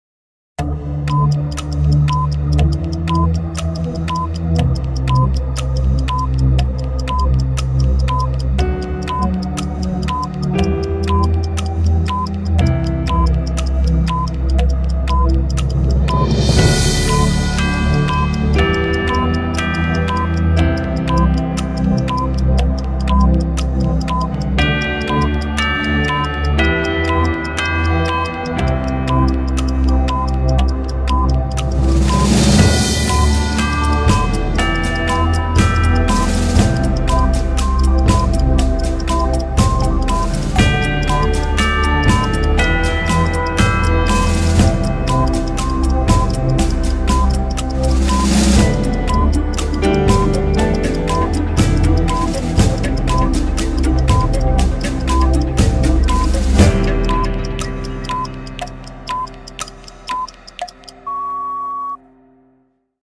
COUNTDOWN TIMER 60 sec ( v 570 ) 1 min timer in 8k with sound effects!
8k ( 7680 × 4320 ) timer 60 to 0 + sound 1min Tick-Tock + beep
Super Hi-Vision (スーパーハイビジョン, Sūpāhaibijon; SHV) Ultra High Definition Video (UHDV) UHD-2